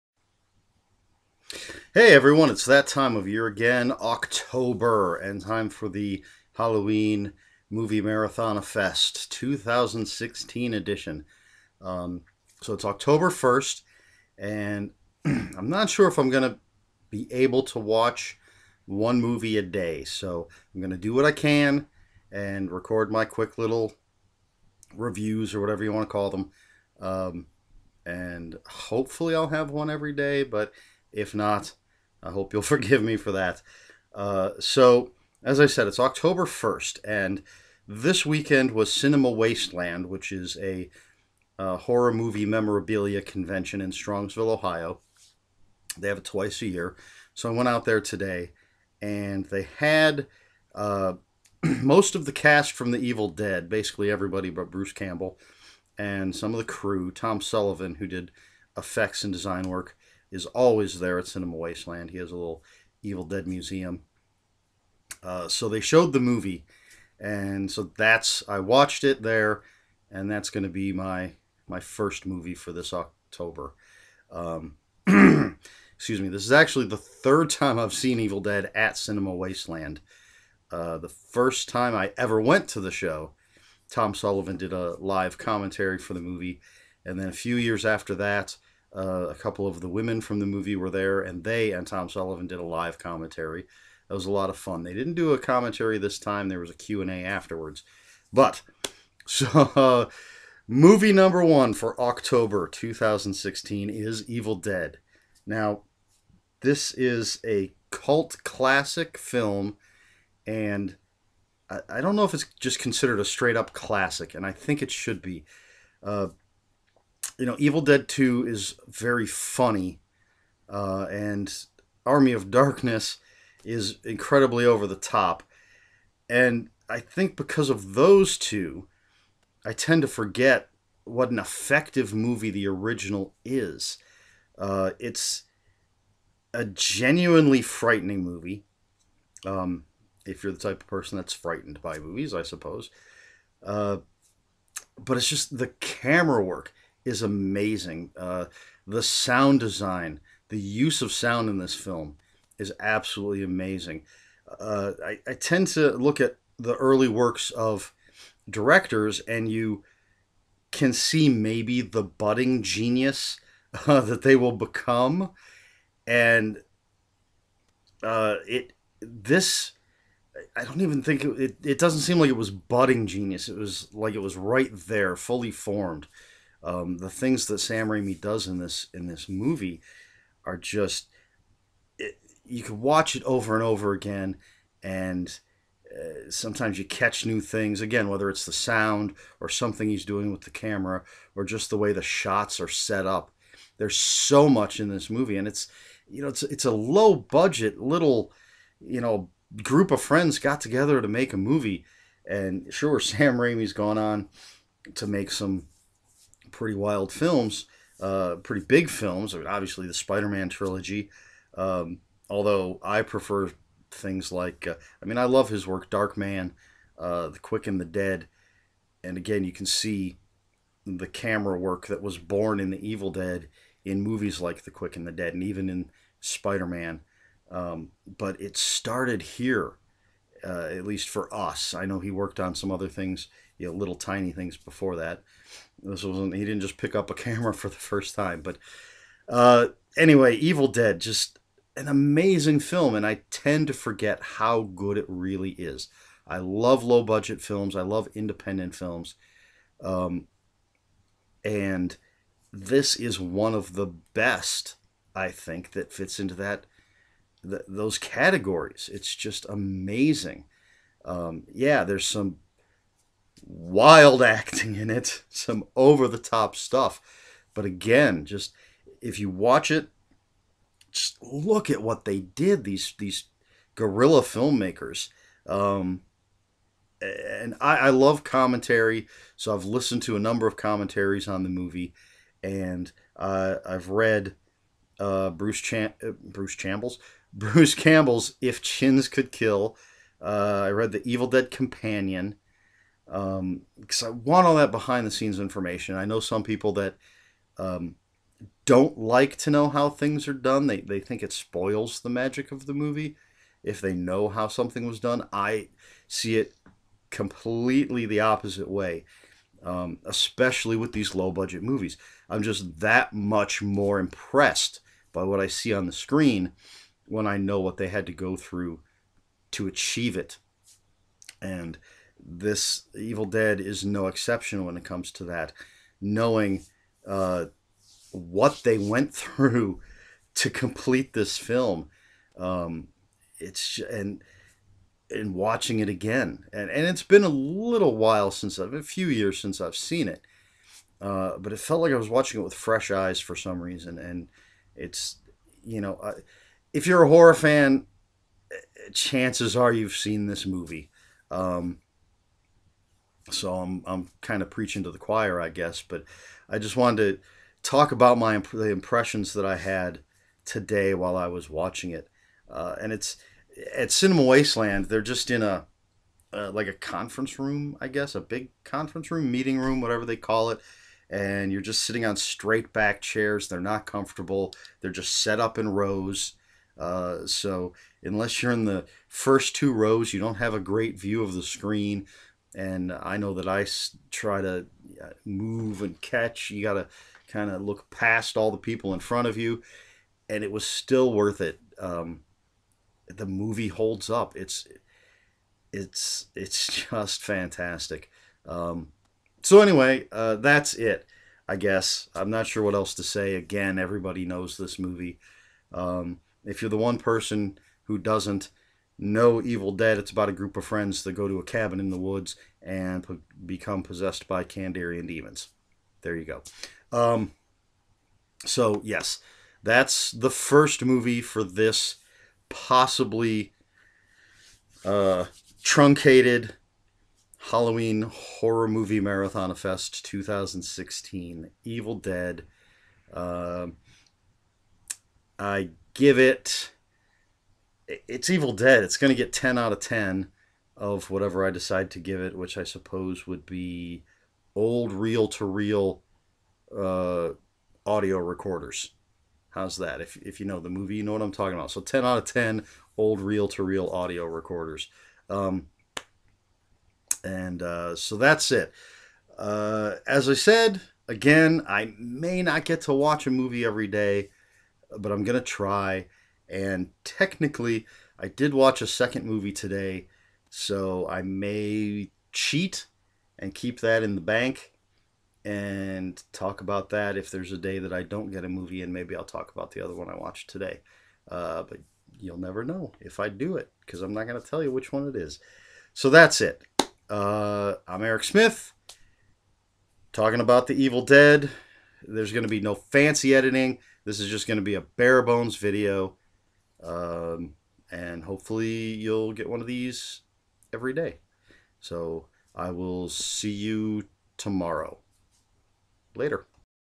Evil Dead/Halloween Movie Marathon-a-fest 2016
It's October 1st, and I'm talking about Evil Dead!